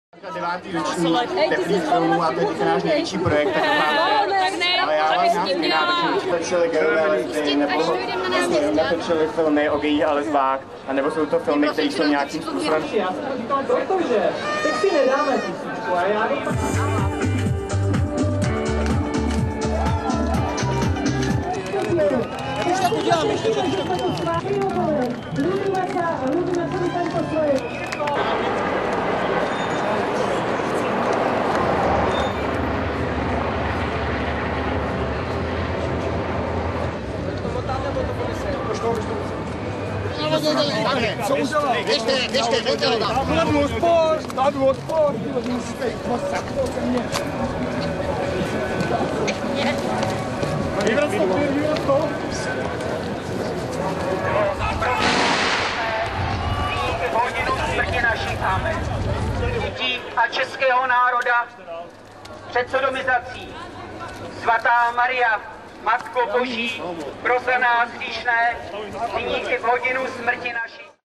Brno - Czech Republic
Gay pride march in Brno and demonstration of czech opposents of homosexuals.